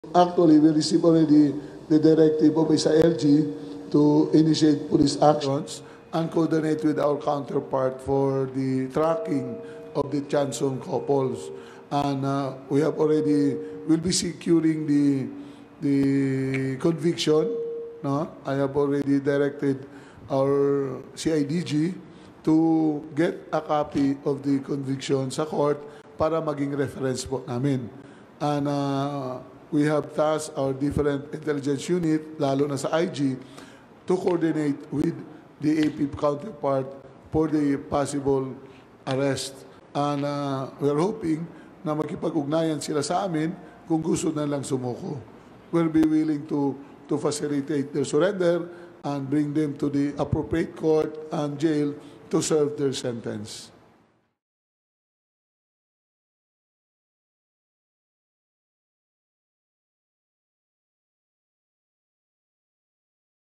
PNP coordinating with AFP to arrest Tiamzon couple
The Philippine National Police (PNP) is coordinating with the Armed Forces of the Philippines (AFP) for the arrest of Benito and Wilma Tiamzon who were recently found guilty of kidnapping and serious illegal detention of three military officers and a member of a narcotics command.